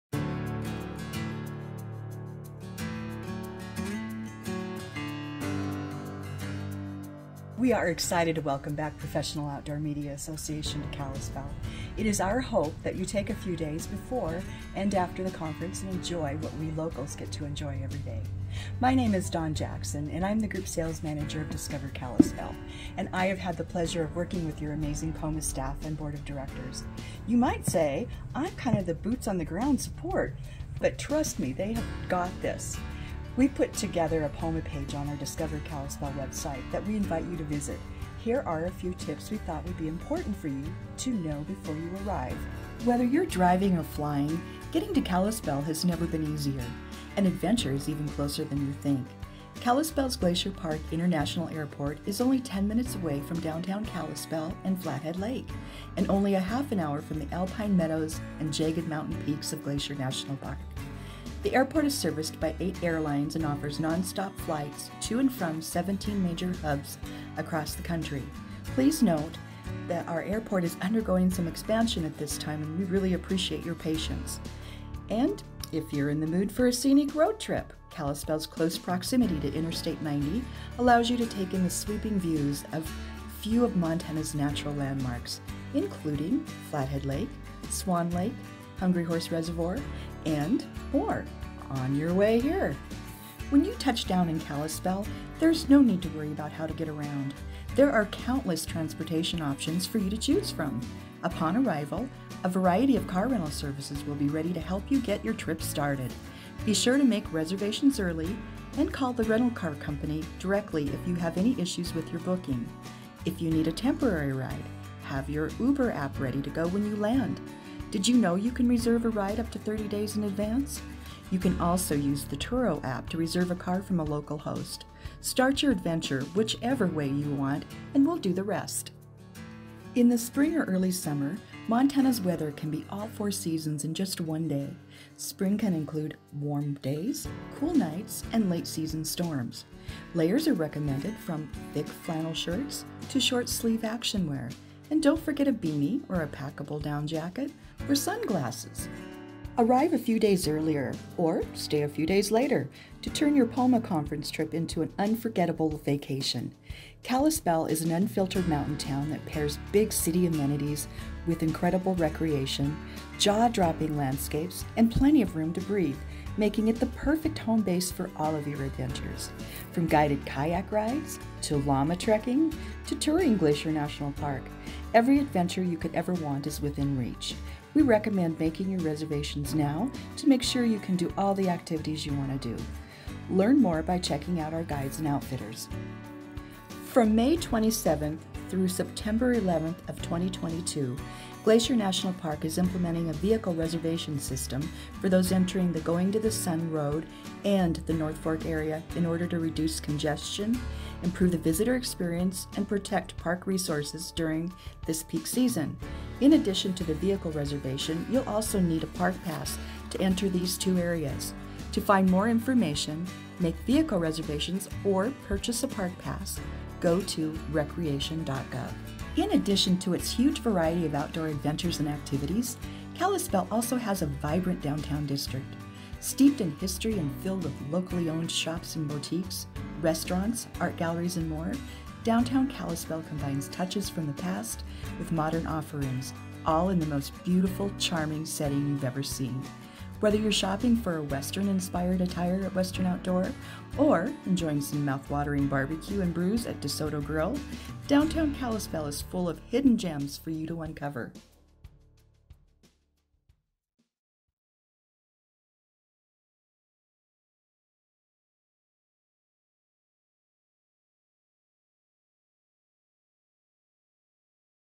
POMA, Welcome Back to Kalispell Montana!
WELCOME BACK POMA! Kalispell is so excited to have you all again.

This video provides tips on how to get here, what to pack, what to do, and where to go.

For additional information on POMA in Kalispell, explore our meeting